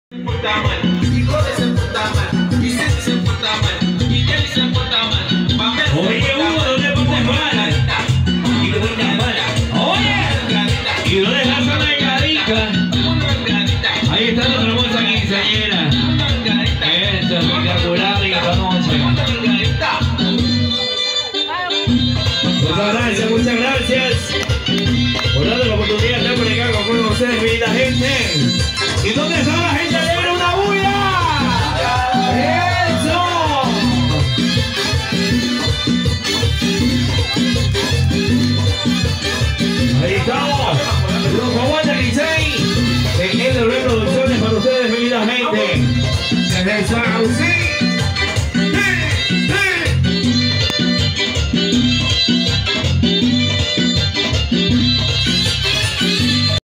Quinceañera.
Amplificando sonido y animación en quinceañera.